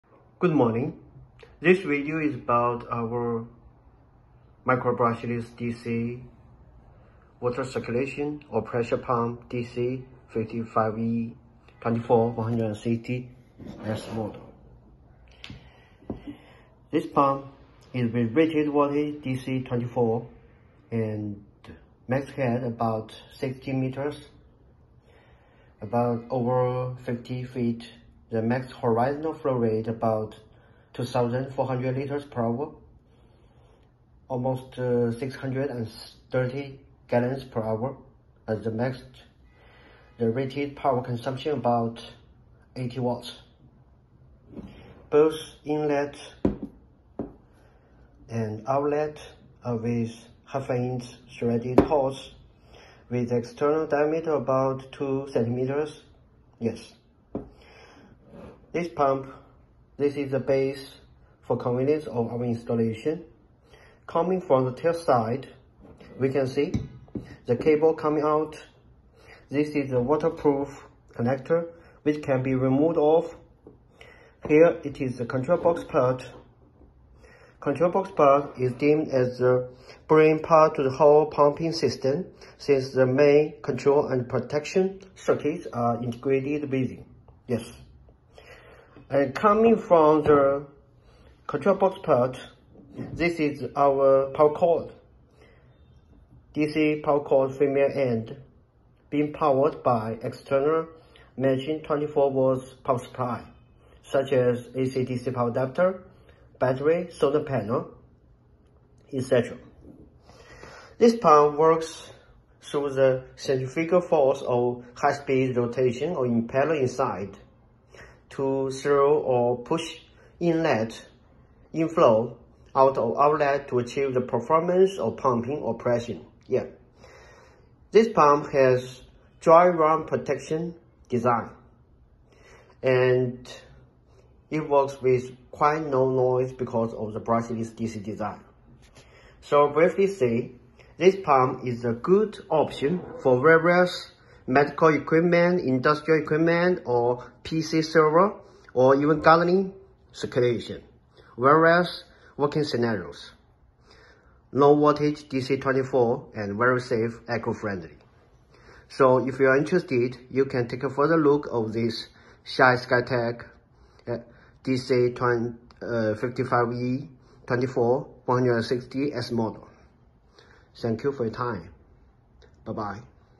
Intro of SHYSKY Brushless DC Circulation Pump DC55E-24160S 24V 80W 52ft Lift 630 GPH Flow Amazing!
This video is to briefly introduce the main parameters and performance of DC55E-24160S pump product of SHYSKY series. Where to Get it? . It is with rated voltage of 24VDC and rated power consumption about 80W. Both inlet and outlet are 1/2'' threaded, with external diameter of about 20mm, which is a normal standard of dimension for pipe connection.

SHYSKY DC55E-24160S Model, Inlet: G1/2, with external diameter of 20mm, outlet: G1/2, with external diameter of 20mm; Max static lift: 16 m (in pressure, 0.16MPa or 23PSI); Max horizontal flow rate: 2000 LPH (~ 38 L/Min).Typical feature is with quite high lift (output pressure) value, but with relatively low flow rate, suitable for some micro pressure system. (Certain parameters, cable length and other specific functions can be specially CUSTOMIZED, based on exact discussion and certain MOQ & lead time.)
Brushless DC motor, also known as EC motor; Magnetic Driven (NO DIRECT AC 100-240V POWER CONNECTION, PROHIBITED); Small size but strong; Low consumption & High Efficiency; Long time continuous working with low noise;Safety - no spark, Explosion - proof; Low noise about 35dB.
Submersible, 100% waterproof; Max Working Temperature: 100 degree; Wide range of working voltage; Maintenance - free; Can be used to pump water, oil, other non-corrosive liquids (special liquids are needed to be pre-tested in advance).
Variety Power: DC electric source, Battery or Solar Panel, etc; Soft Start with Low in-rush current, Especially suitable for Solar SYS who can not provide over-high startup impulse current; Eco-friendly material design; OEM and ODM are accepted based on MOQ and exact inquiry of customers.
NEW DRY-RUN PROTECTION feature is integrated in default: Pump works at rated voltage, when no or in-sufficient water in-flow (or significant air bubble contained), it is DRY RUN scenario and protection will be triggered; Pump will access stop-run-stop-run---cycle, greatly reducing rotation. When flow comes sufficiently again, pump will re-access normal run. This greatly reduces physical damage possibly caused by friction between parts, and generated heat too, protecting pump. Yet, protection does not completely eliminate the impact if long time dry run, since pump doesn't stop completely in this context.